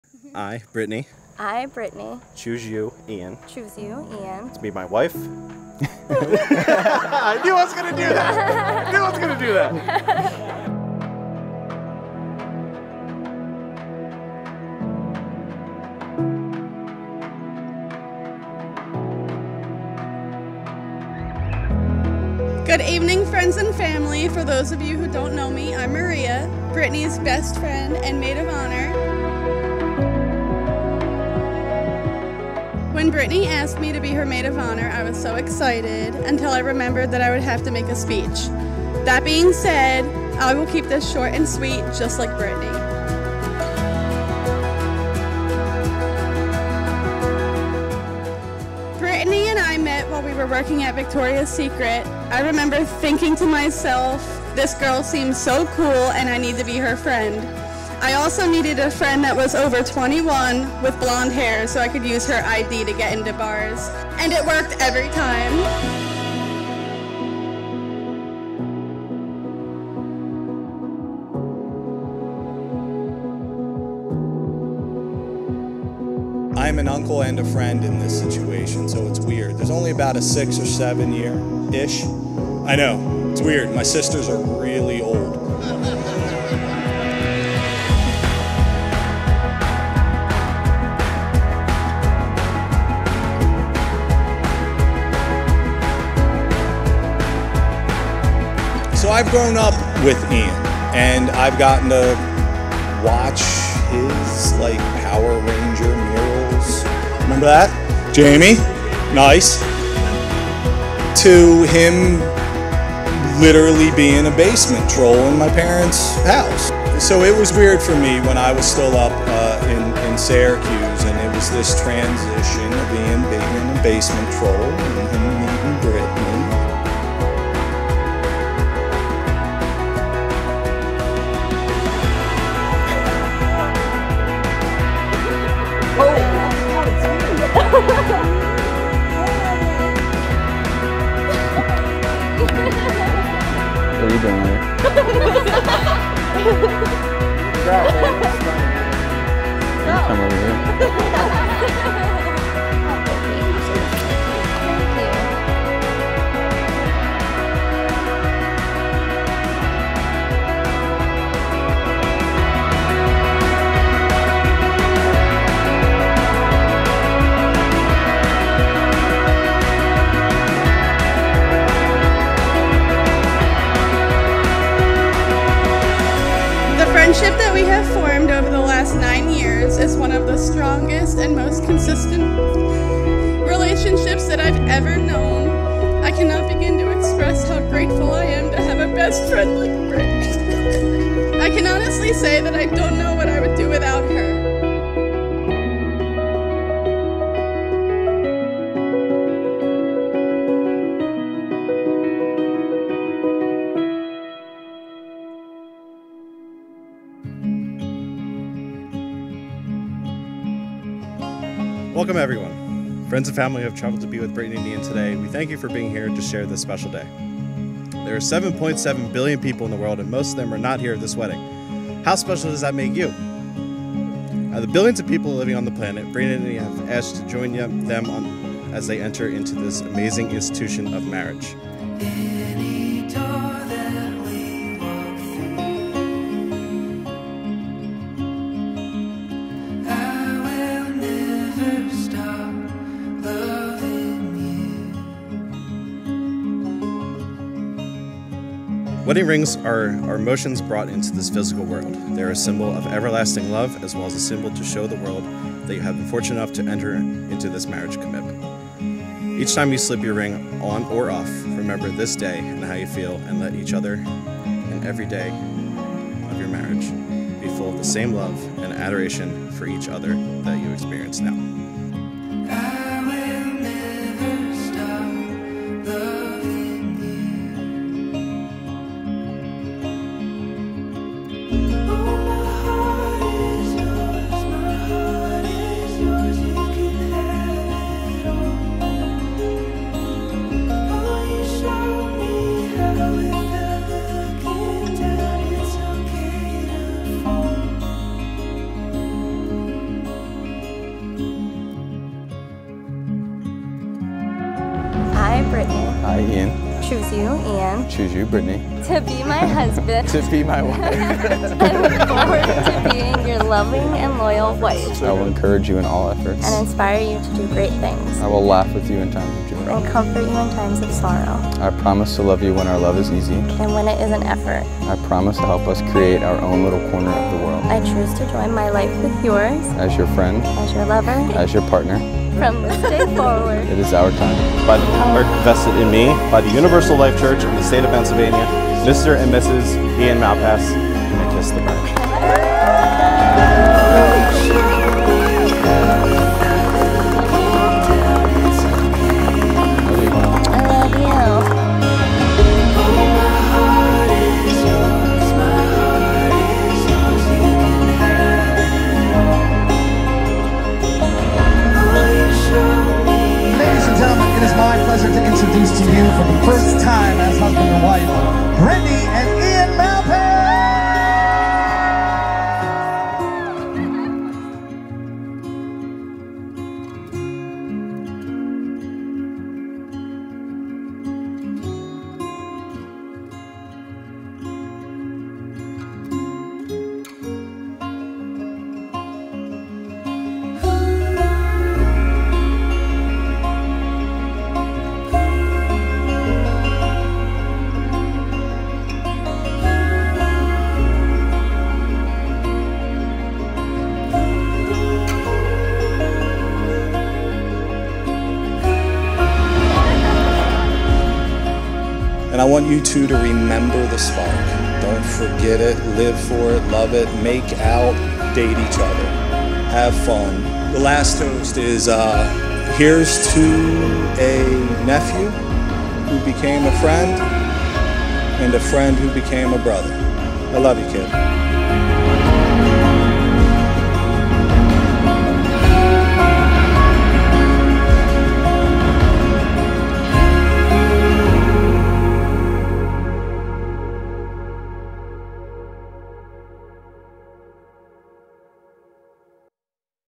Pennsylvania Wedding | Glenmoore
The wedding film of Ian & Brittany Malpass

This was the first wedding I booked when I decided to start my business a year ago. After many weddings as lead and second shooter for other videographers and two previews weddings of my own, the wedding day of Ian and Brittany came and this was the result. Enjoy!!!
Photographer |@heatherb_photography
Makeup | @kat_reboh
Cake | @sweetirenescakes
Music | @vinnyderenzis
Caterer | Experience DBESS LLC (FB)
Cinematography: @oscar.alexis.rivera